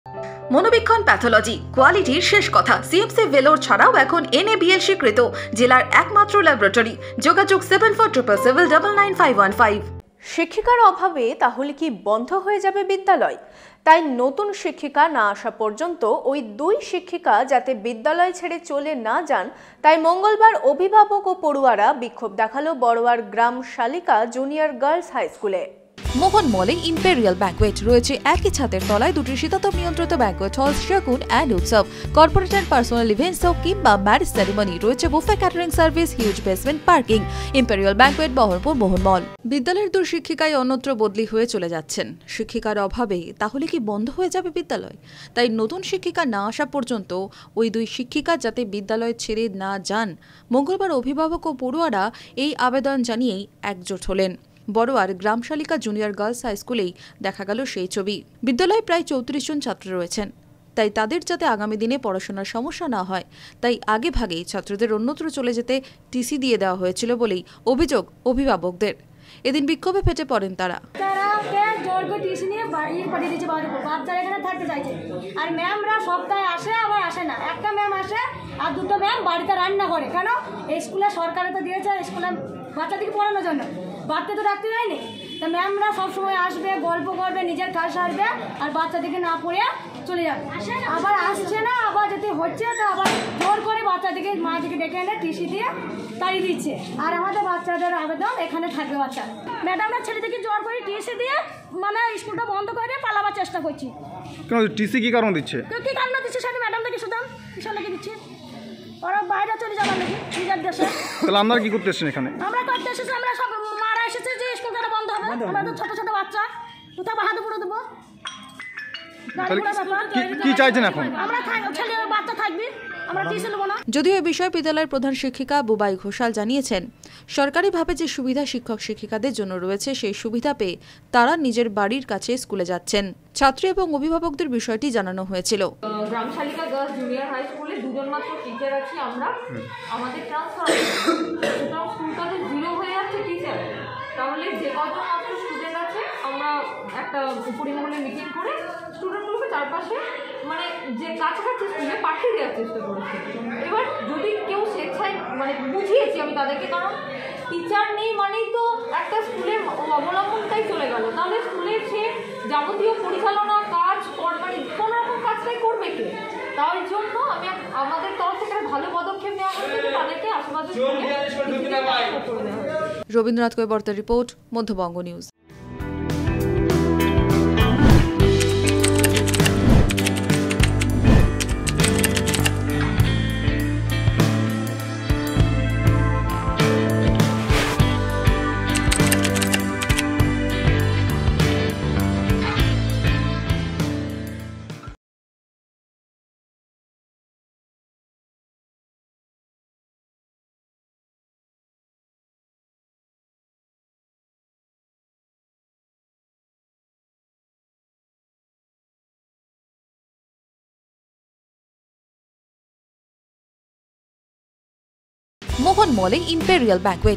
শিক্ষিকাদের বদলি, প্রতিবাদে বিক্ষোভ অভিভাবক পড়ুয়াদের
বিদ্যালয়ের দুই শিক্ষিকাই অন্যত্র বদলি হয়ে চলে যাচ্ছেন, শিক্ষিকার অভাবে তাহলে কি বন্ধ হয়ে যাবে বিদ্যালয়। তাই নতুন শিক্ষিকা না আসা পর্যন্ত ওই দুই শিক্ষিকা যাতে বিদ্যালয় ছেড়ে চলে না যান তাই মঙ্গলবার অভিভাবক ও পড়ুয়ারা বিক্ষোভ দেখাল বড়ঞার গ্রাম শালিকা জুনিয়র গার্লস হাইস্কুলে।